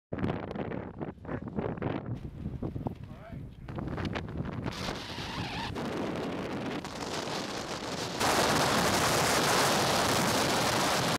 Wind Prayers Answered
Ivanpah dry lake @ IBX2017
Gusts 60+ mph